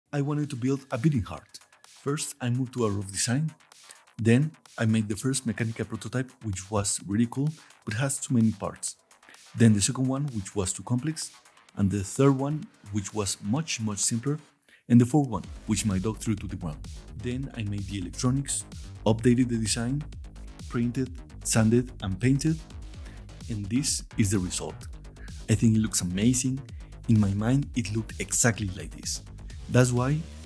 How To Build A Mechanical Heart
I built a mechanical heart as a gift. Turned out awesome.
3D printed and it actually beats! Simple design and some mechanics for the beat!